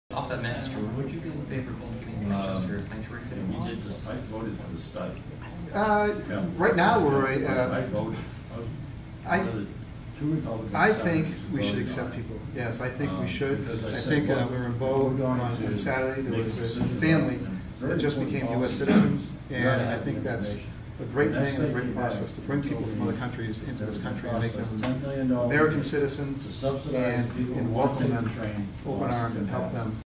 Kevin Cavanaugh comments on making Manchester a Sanctuary City
Last night at the League of Women Voters Candidate forum, Manchester Alderman and State Senate candidate Kevin Cavanaugh reiterated his support for making Manchester a sanctuary city. When asked by a Bow voter if he believed that Manchester should become a sanctuary city, Cavanaugh responded, “I think we should accept people. Yes I think we should…that’s a great process to bring people into this country and to make them American Citizens.”

NH GOP Chair Jeanie Forrester released the following statement regarding Cavanaugh’s comments:

“Imposing sanctuary city status on Manchester or any other city or town in New Hampshire is both foolish and dangerous. Sanctuary cities from Baltimore to Chicago have seen consistently higher rates of violent crime than non-sanctuary cities. This is on top of the potential loss of federal funding that will almost certainly accompany the designation. Cavanaugh’s proposal risks both the safety and well-being of every man, woman and child in Manchester and throughout the state.”

 “We simply cannot put our children at risk for Kevin Cavanaugh’s short-term political gain.”